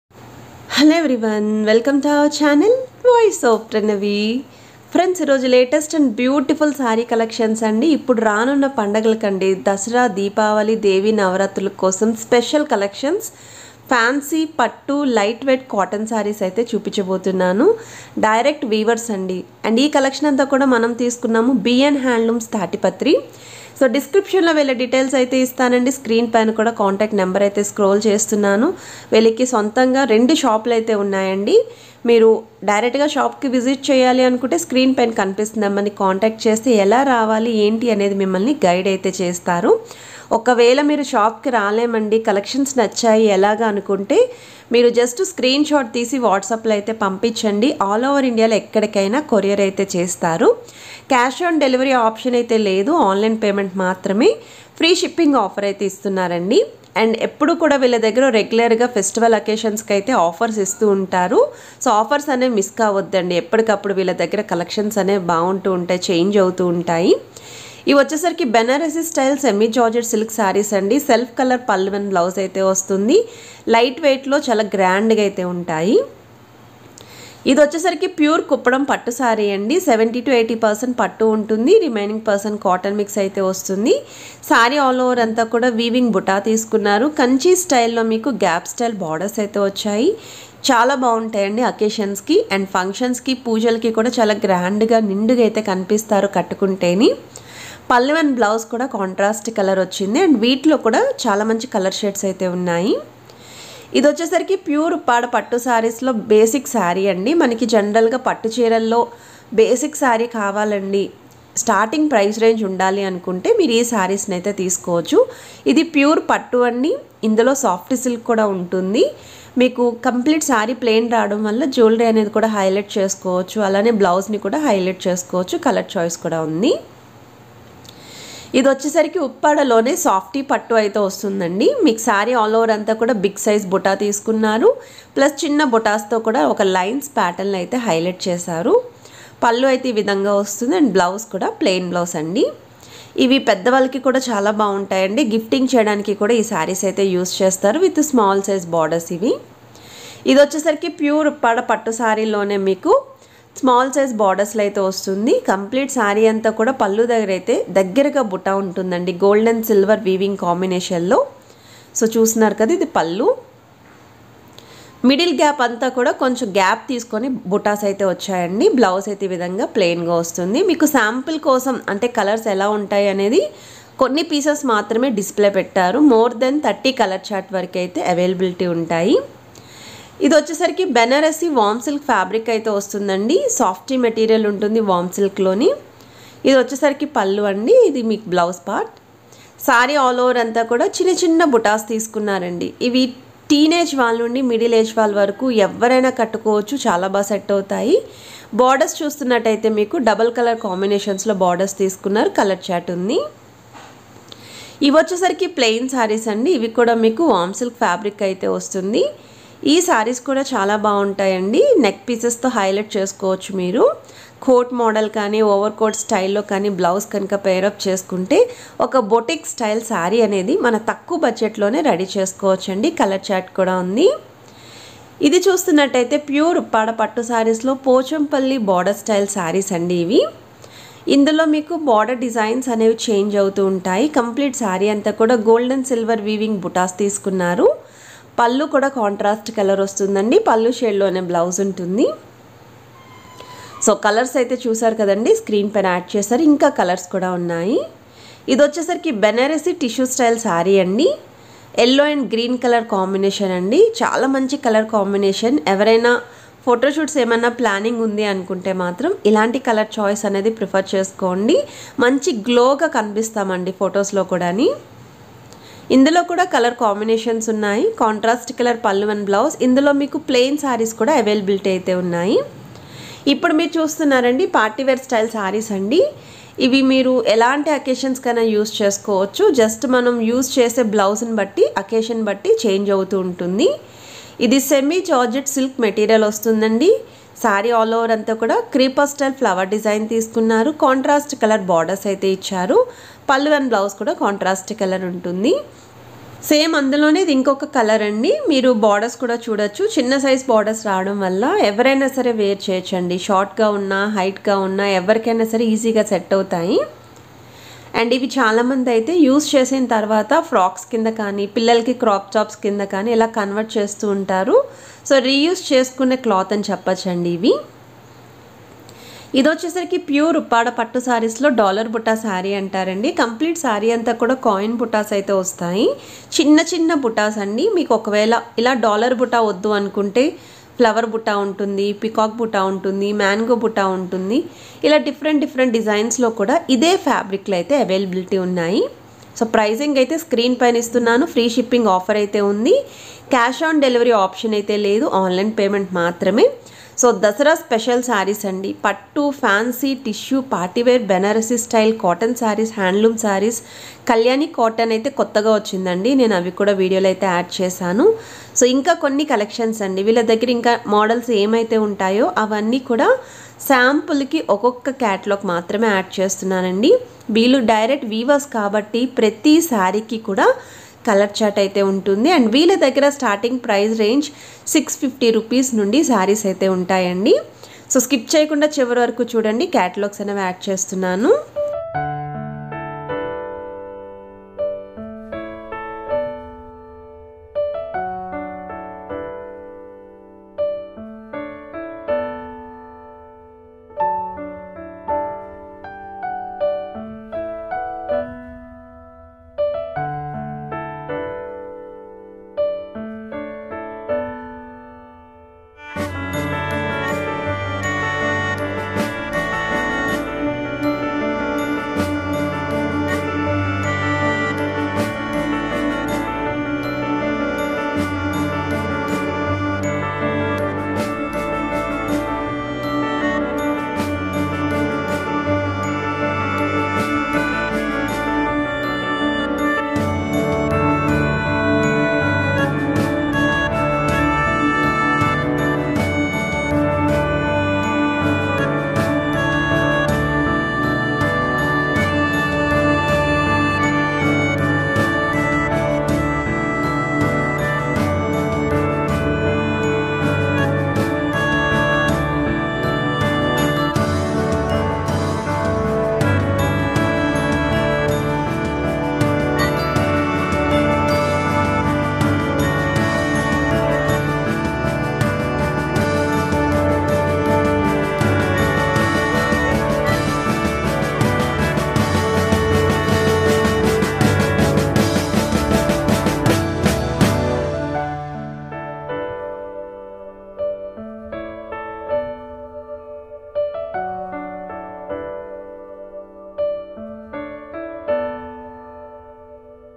Dasara Special Uppada Kuppadam Saree నేరుగా తయారీ దారులు నుండి Wholesale ధరలకే సింగిల్ శారీ
Address :
Satyanarayana Handlooms
Tatiparthi
Gollaprolu Mandal
East Godavari District
Andhra Pradesh
Pin : 533445